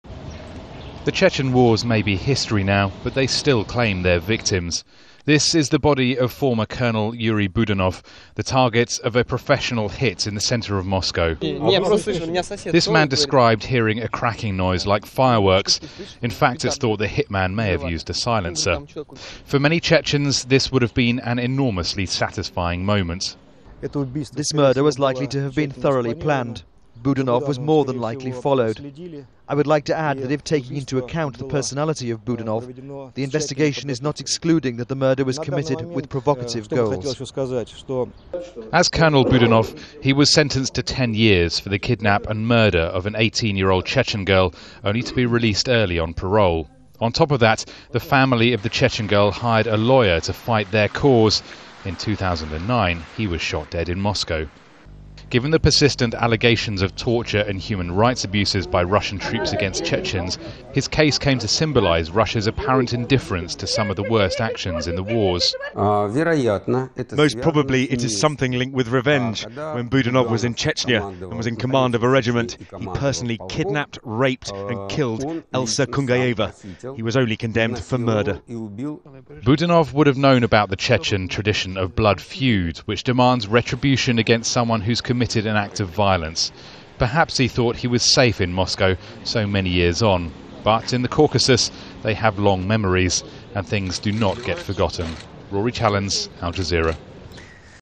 Russian war criminal assassinated in Moscow‎
A Russian army colonel convicted of murdering a Chechen teenager has been killed in a daylight shooting in Moscow. Yuri Budanov was Russia's only convicted war criminal.

Budanov was sentenced to 10 years in jail for kidnapping and murdering the 18 year-old in the year 2000 but he was released on parole in 2009, sparking protests in Chechnya.

Given the persistent allegations of torture and human rights abuses by Russian troops against Chechens his case came to symbolise Russia's apparent indifference to some of the worst actions in the wars.

While the Chechen wars may be history now, they still claim their victims.

Al Jazeera's Rory Challands reports.